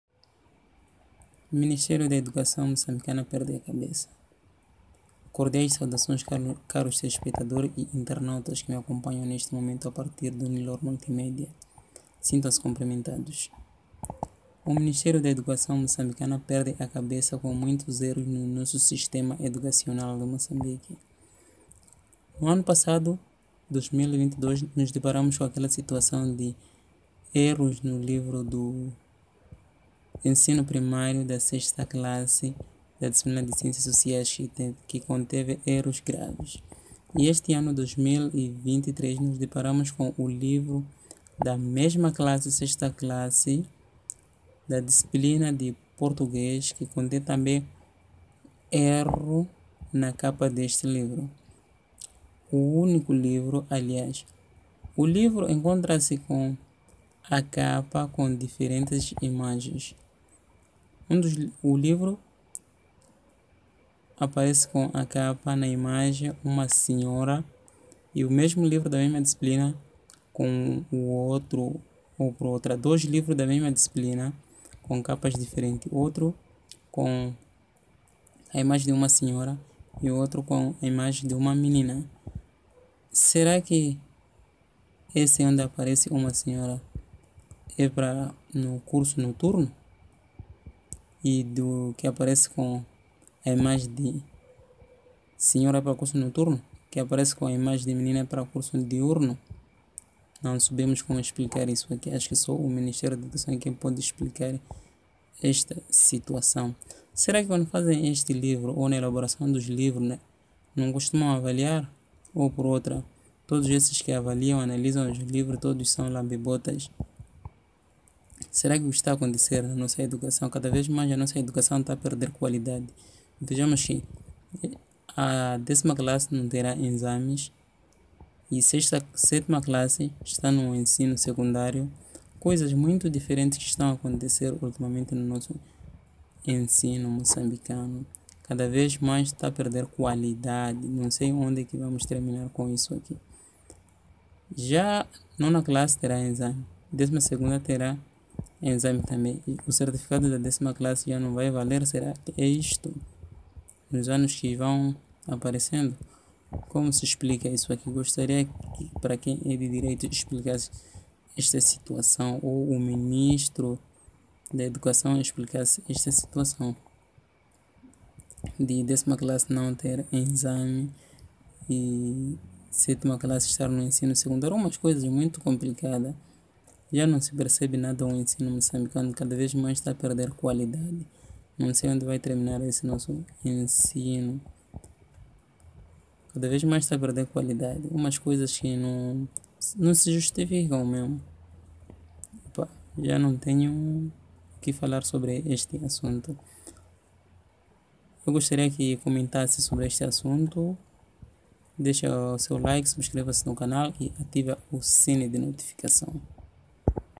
Este Ano 10°Classe Não Tera Exames
O Sistema Da Educação de Mocambique Modo do melhor pra no erado